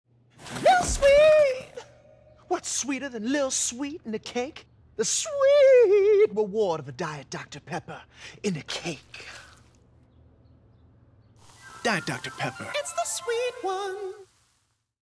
Dr Pepper - Lil' Sweet: Cake surprise (2018)
Since 2015, Justin Guarini has been starring as Lil' Sweet in Diet Dr Pepper television commercials. He is an American singer, musician, actor, host and record producer, who in 2002 was the runner-up on the first season of American Idol.

Although the character of Lil' Sweet is not gay per se, he acts as a free spirit, not afraid to embrace his feminine side. His speech and the accessories he uses (like a little pink car) allude to "the rainbow".